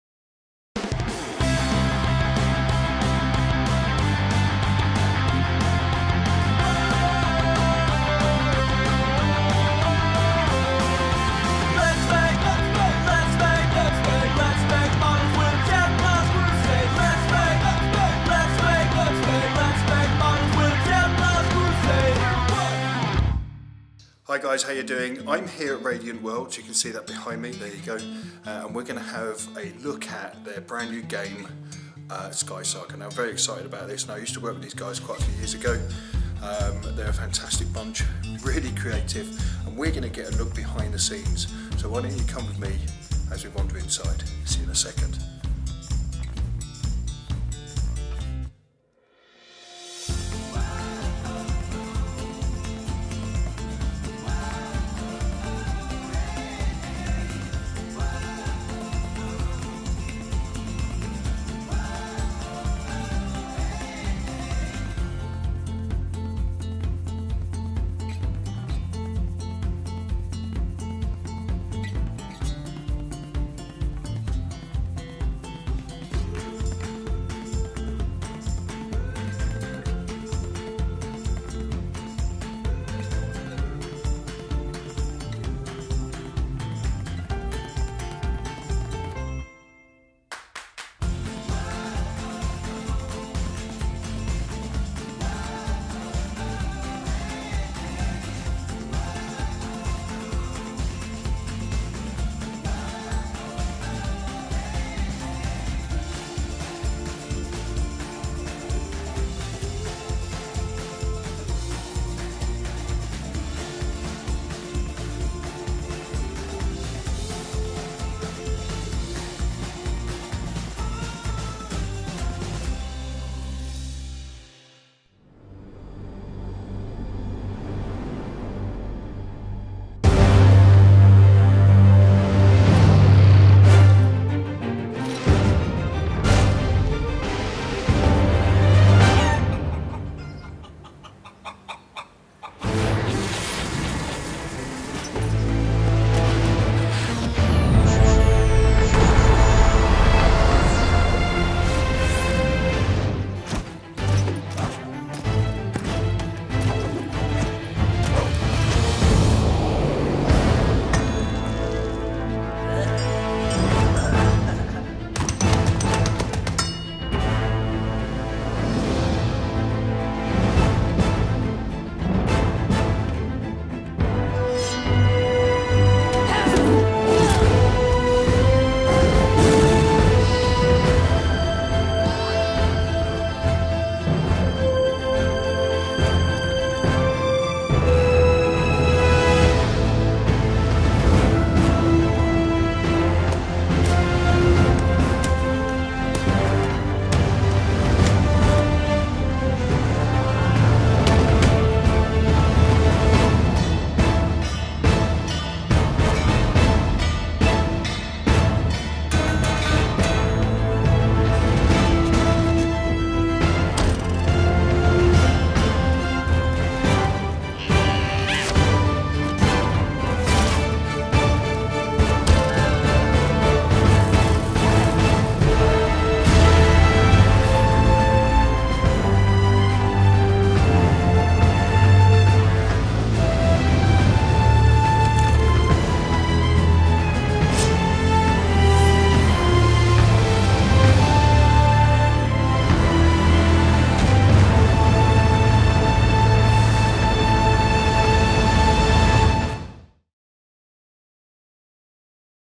SkySaga #1 Exclusive!
Have you ever wondered how they put computer games together? Or wondered if you could get a look behind the scene and hear how the designers go about getting into computer games? Well in this series we get to do that as we follow skysaga!

Music owned by Myself, created for me, for my channel.
Music Licensed and Paid for from Premiumbeat.com